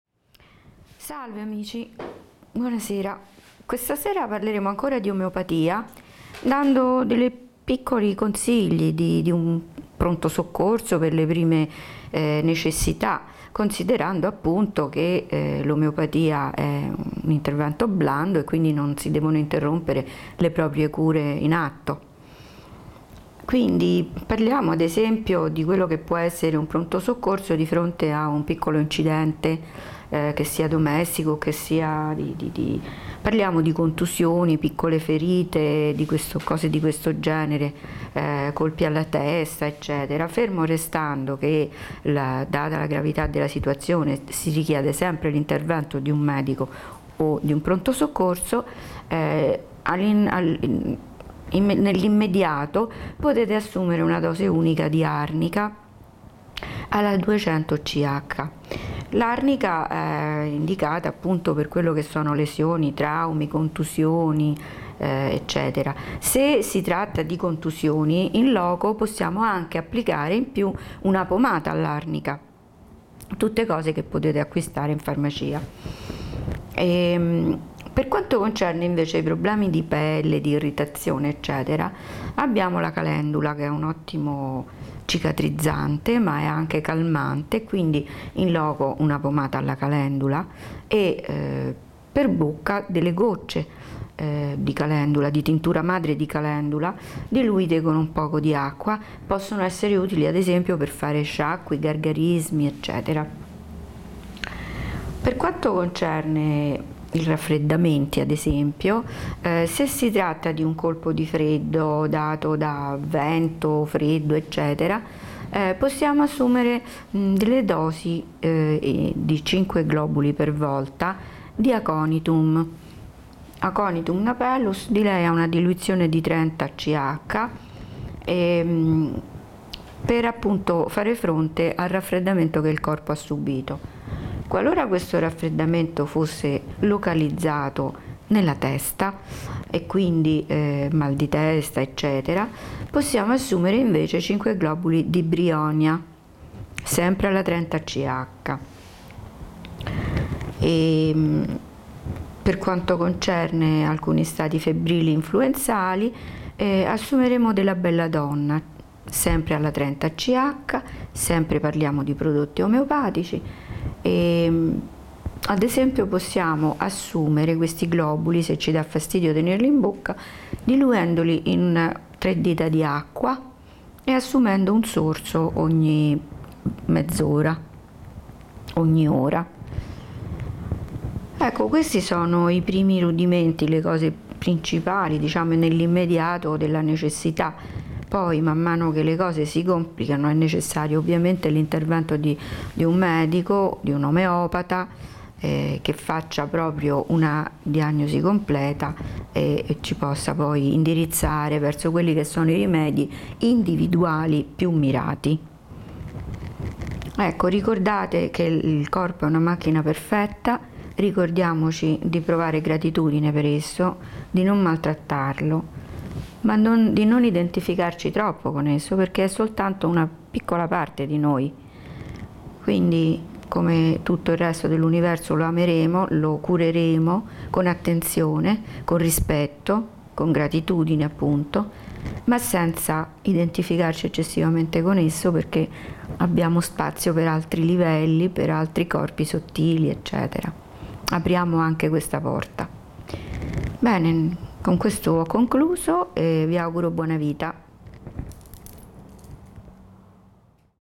Evolvenza Omeopatia Manuela Schneider
- L’efficacia delle cure alternative è data anche dalla convinzione del malato che esse siano efficaci.
- The effectiveness of alternative treatments is also given by the patient's belief that they are effective.
- La eficacia de los tratamientos alternativos también se hace por la creencia del paciente de que sean efectivos.
- L'efficacité des traitements alternatifs est également donnée par la croyance du patient qu'ils sont efficaces.
Evolvenza.it